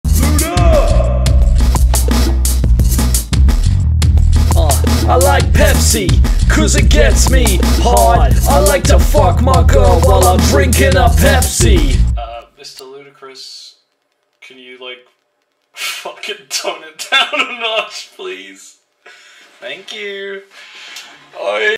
ludacris pepsi sponsor
In 2002: American rapper and actor Ludacris became embroiled in controversy on account of his sexually explicit lyrics, littered with profanities and references to illegal drugs. PepsiCo then gave him 2 Pepsis, failing to handle the resulting backlash.

The rest, as they say, is history.